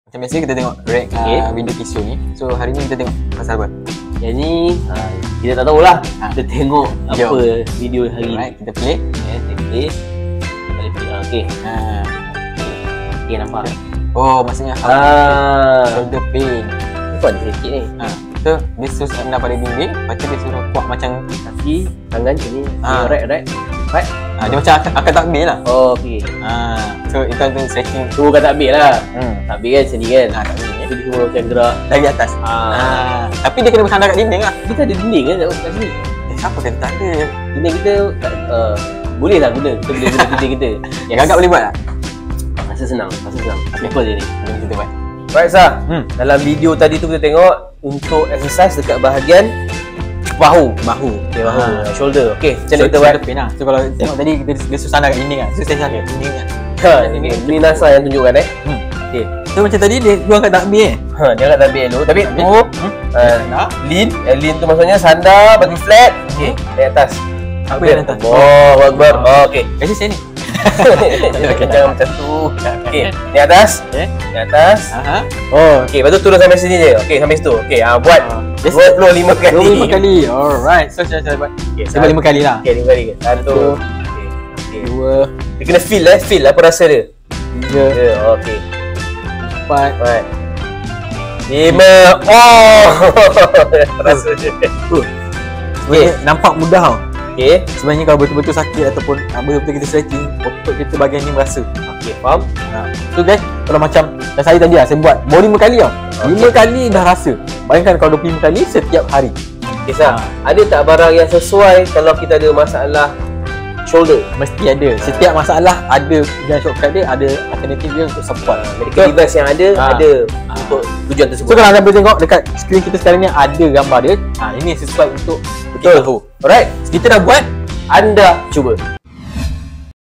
VIDEO REAKSI CUBA SENAM - EPISODE 2
Ini adalah reaksi terhadap video senaman ringkas yang diajar oleh pakar atau fisio yang boleh mengurang kesakitan yang sedang anda alami dan produk medikal yang sesuai dan boleh digunakan.
Tekan butang 'LOCENG' dan ON NOTIFICATION supaya anda mendapat notifikasi mengenai kandungan baru daripada kami :)